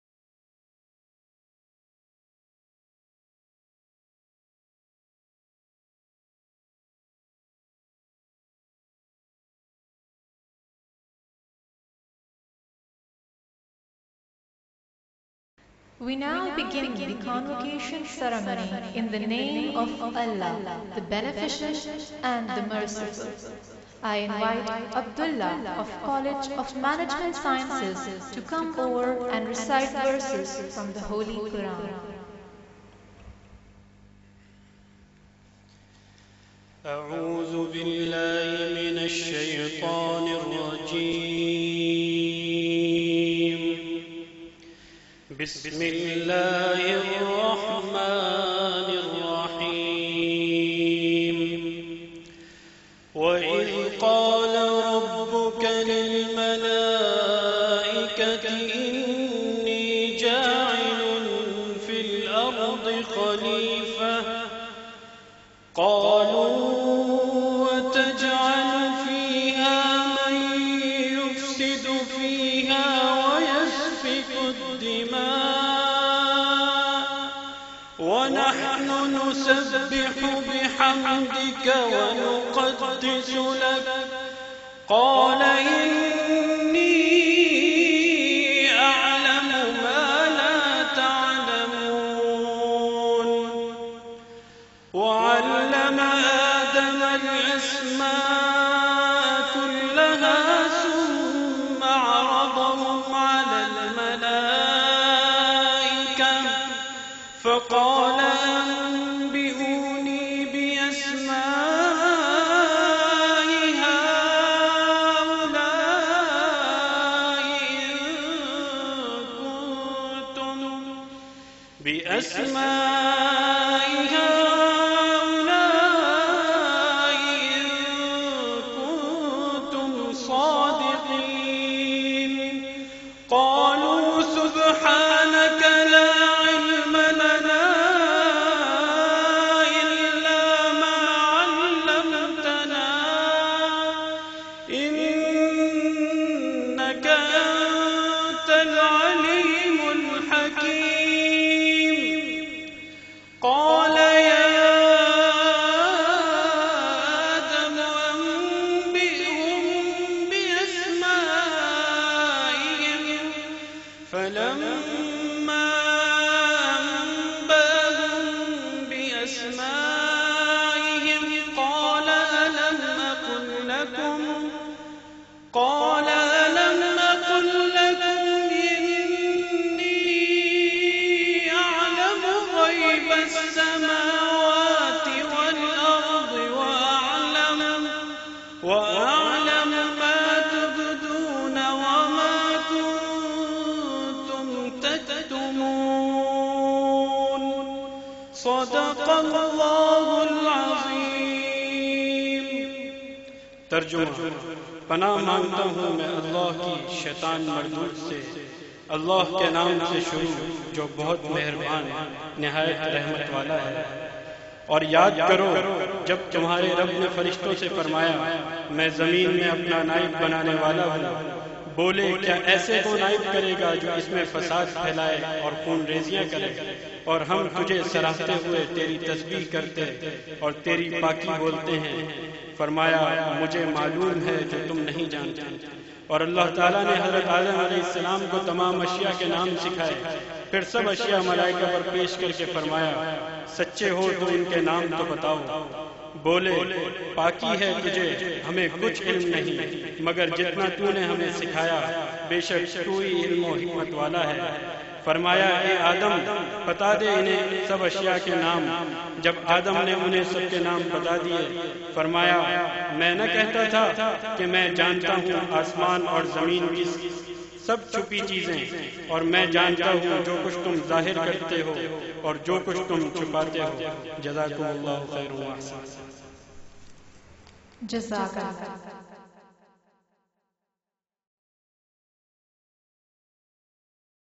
Recitation of Holy Quran : KIET Convocation 2014
12 Convocation Ceremony 2014 of KIET was held on 26th April, 2014 at PAF Museum Convention Hall, PAF Base Faisal. Mr. Abdullah of College of Management Sciences recited the verses from Holy Quran.